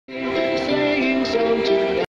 Road signals || project || by class 1st students || Alfa convent school
Second science exhibition 2108 alfa convent school salon raebareli dilshad sir almeen iqra sonali principal shuaib ahmad khan sir